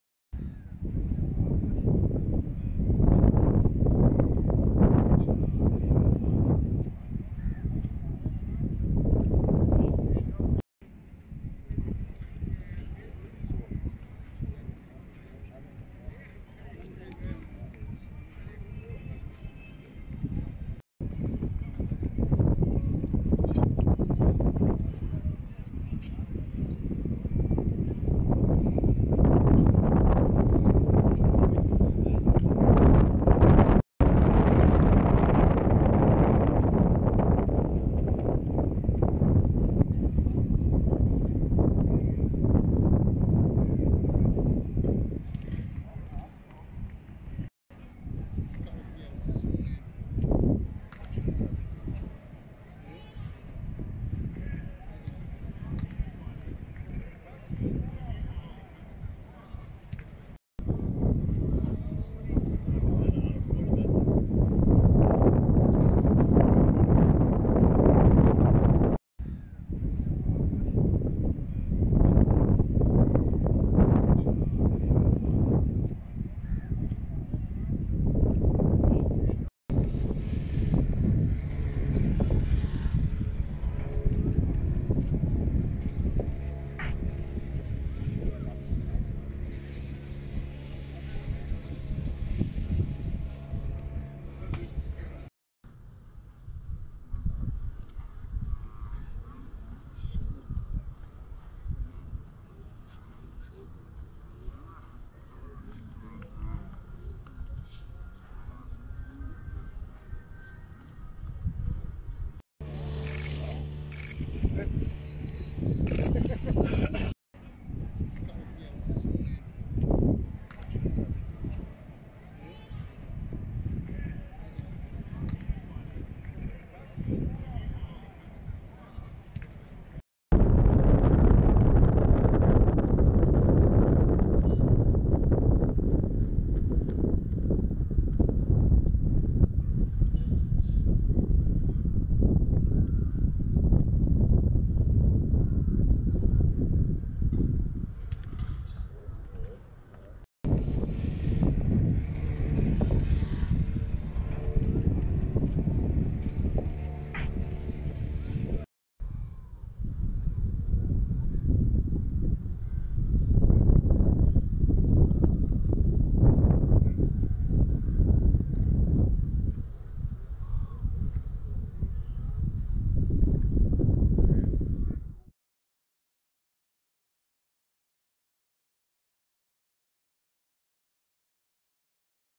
بعد واقعة إريكسن.. جهاز الصدمات القلبية المتحرك يظهر في مباراة الزمالك والمقاصة
تصوير : نادر نبيل